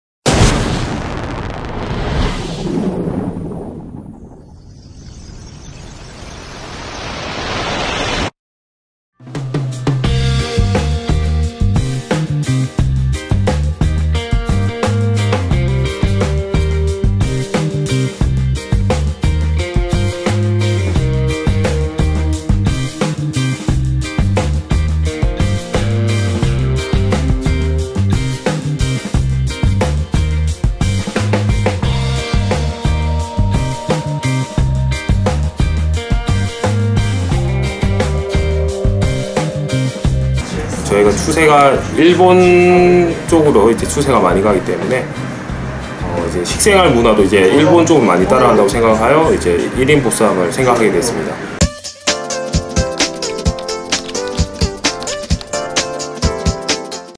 [데일리팝TV-혼밥 탐방] 다양화 되는 혼밥 메뉴 1인 보쌈 '싸움의 고수'
혼밥 문화가 대중화되고 있는 추세에도 불구하고 여전히 혼밥하기 힘든 메뉴들도 있다.

이를테면 혼밥 레벨에서도 고단계에 속하는 혼자 고기를 구워먹거나, 패밀리레스토랑을 방문하는 것이다.

이와 더불어 족발과 보쌈의 경우도 혼자 먹기 힘든 메뉴에 속한다. 보통 小, 中, 大 형태로 판매되는 보쌈을 혼자 먹기에는 양도 많고 가격도 비싸기 때문이다.

이같은 고충을 해소하고자 '1인 보쌈'을 전문으로 취급하는 '싸움의 고수'가 등장해 보쌈 매니아로부터 호응을 얻고 있다.